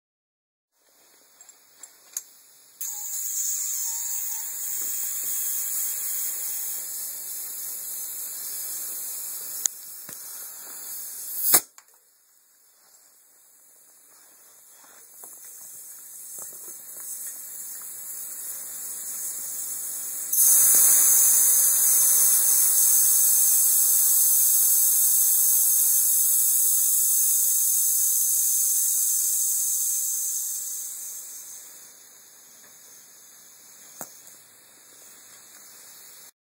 air suspension.wmv
Air suspension on my 1969 Karmann Ghia Pan